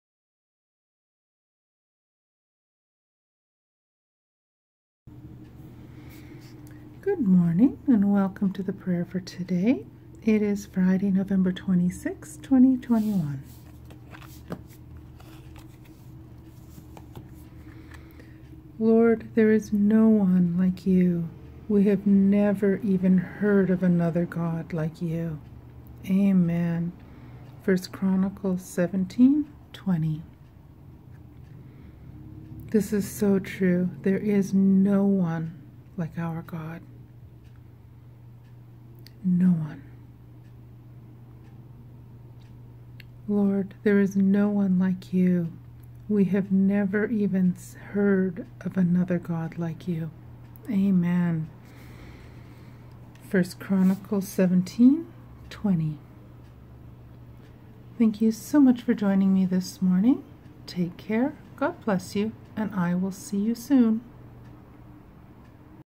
Prayer of the Day - November 26th 2021
Please join me for a quick prayer for the day ♥  This comes from the 101 Best-Loved Bible Prayers set by Christian Art Gifts.

♥ Karen